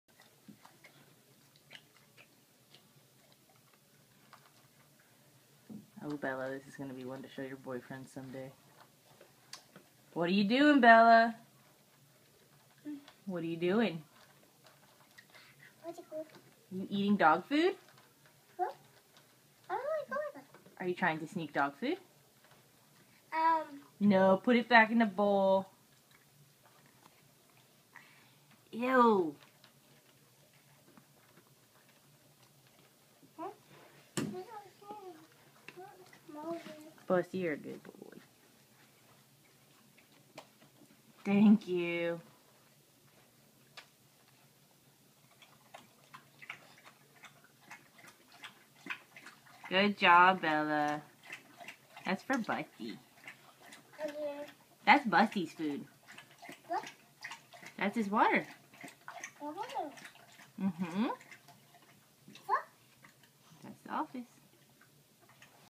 Stealing her puppy's food
Bella sneaking busters food